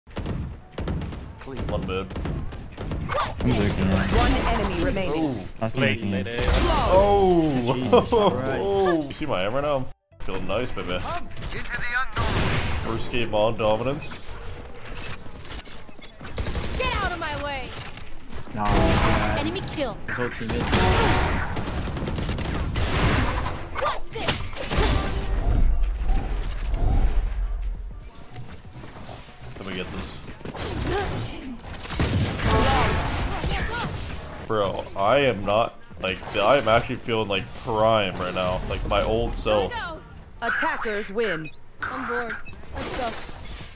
PROD shocks himself & make Enemy SURRENDER with this...
Prod is back to his prime when he can dominates enemy with just 23 kills but only 4 deaths. His Jett with OP is back. The king is back!!! That 4K is way too dirty. After that, enemies just surrendered the game.